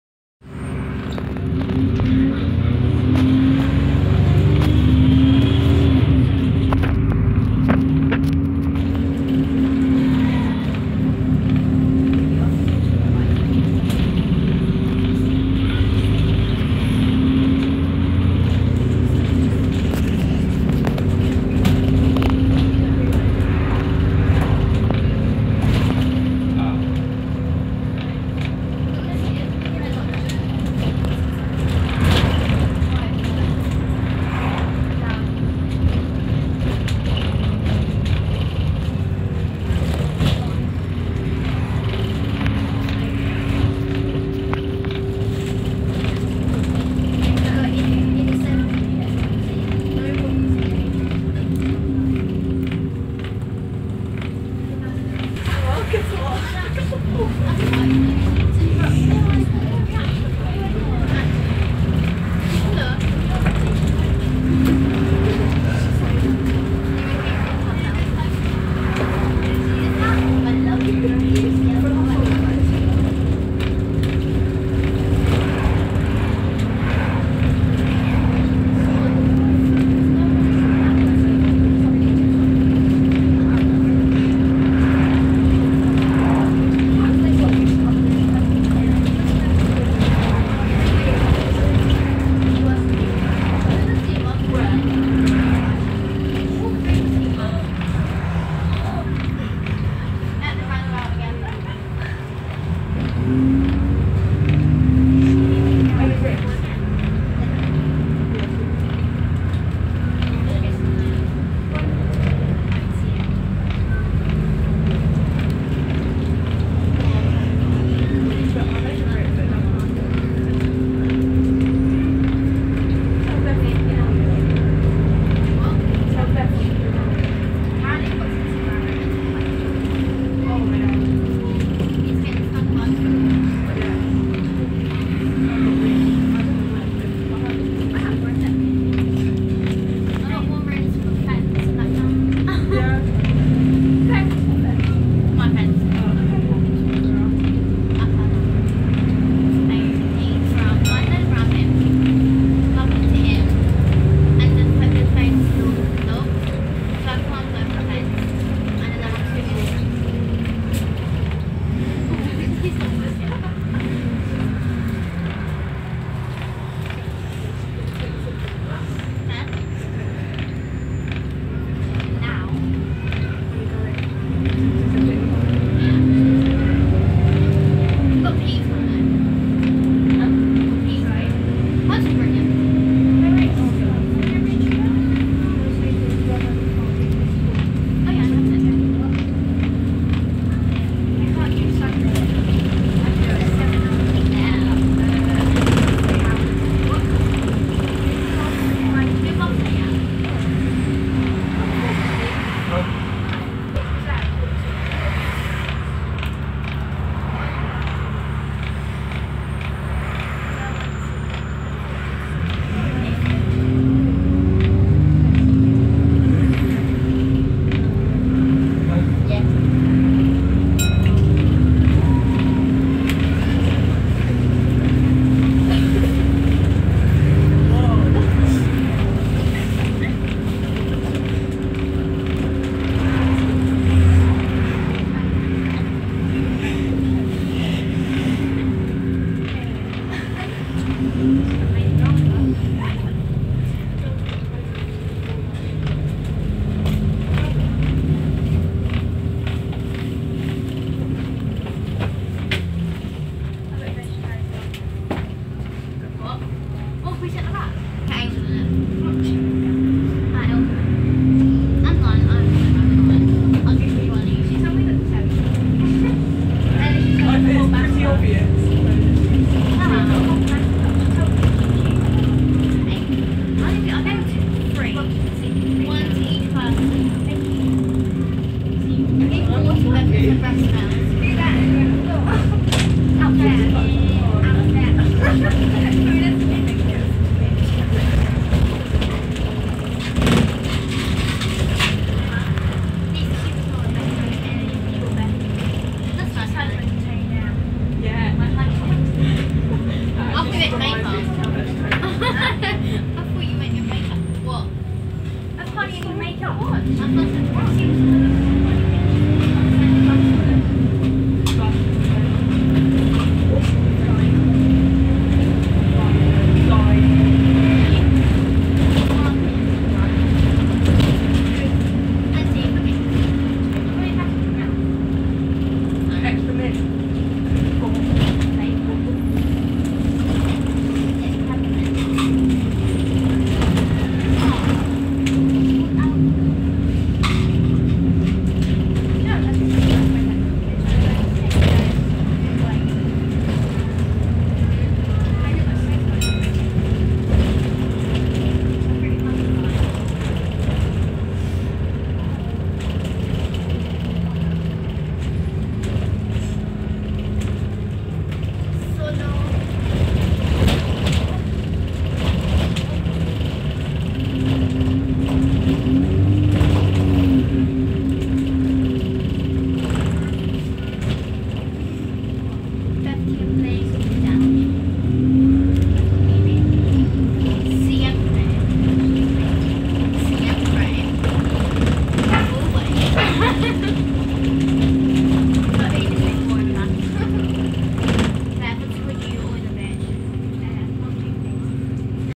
Stagecoach in Eastbourne 19648 SP60 DTF
Filmed on its normal allocated rout to Hampden Park 18/9/17.